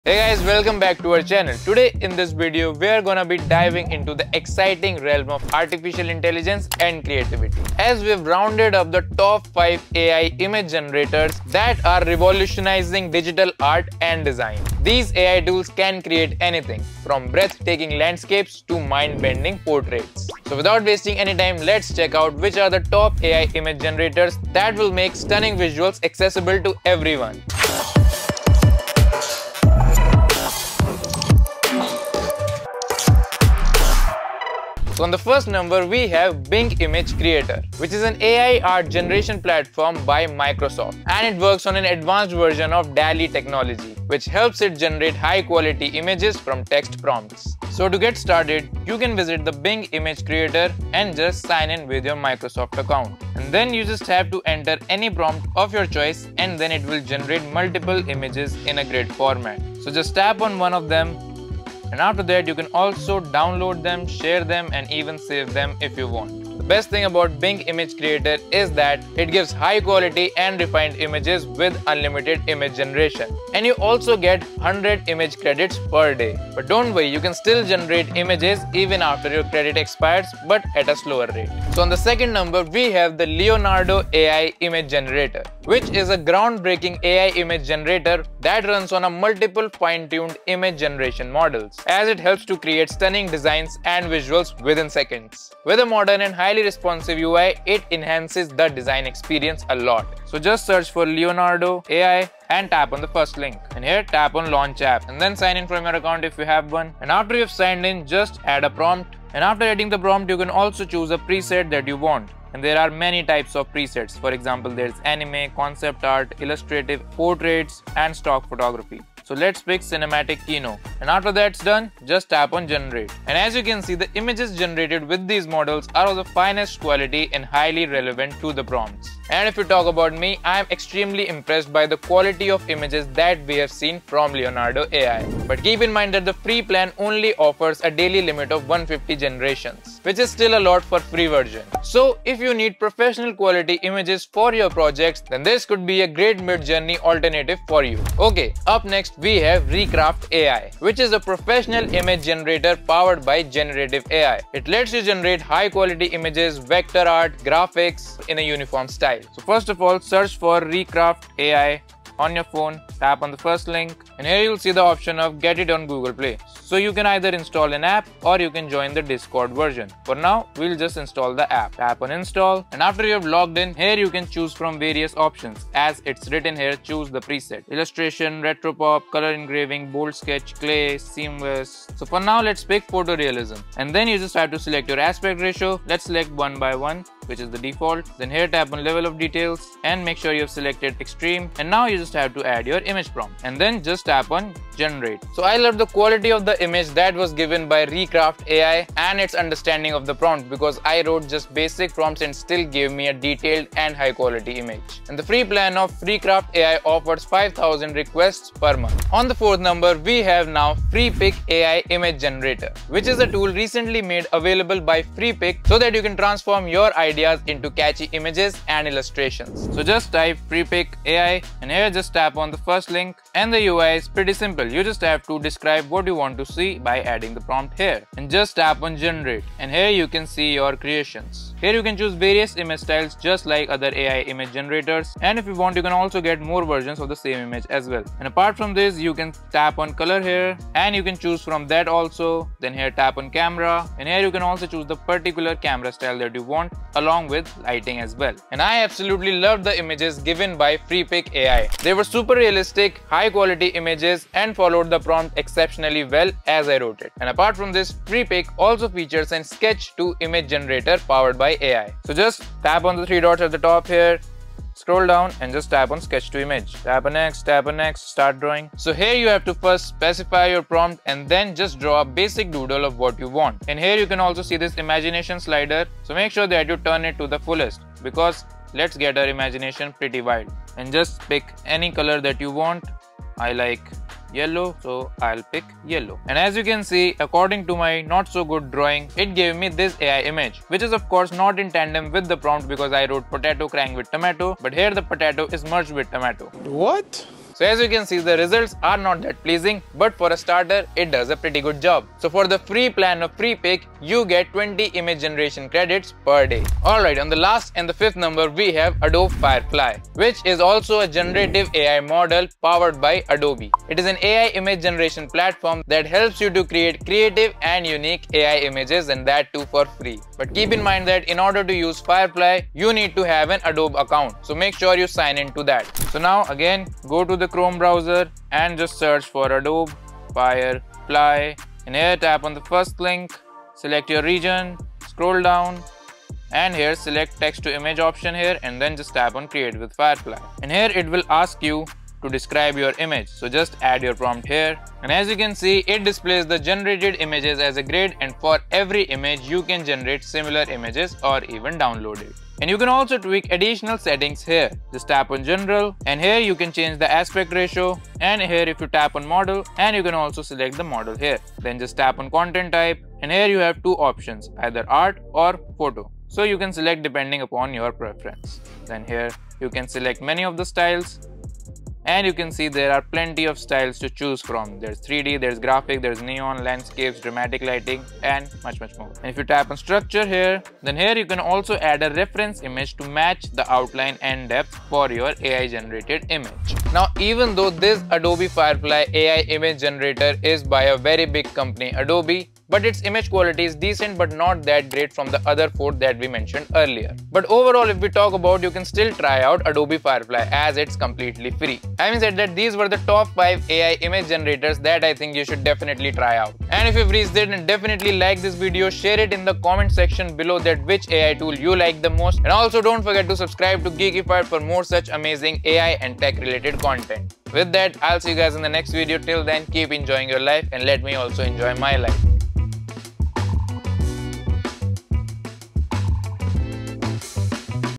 Best Free AI Image Generators in 2024: Tested (Don't Miss Out!)🔥🔥
In this video, I tested the best AI image generator tools to create high-quality images using text-to-image AI. Watch the entire video to find the tool that best suits your needs.

TIMESTAMPS :
0:00 - Best Free AI Image Generators in 2024
0:36 - Bing Ai Image Generator
1:24 - Leonardo Ai Image Generator
2:31 - Recraft Ai Image Generator
3:31 - Freepik Ai Image Generator
4:10 - Adobe Firefly Image Generator

Top Recommendations ✅

Gears we use :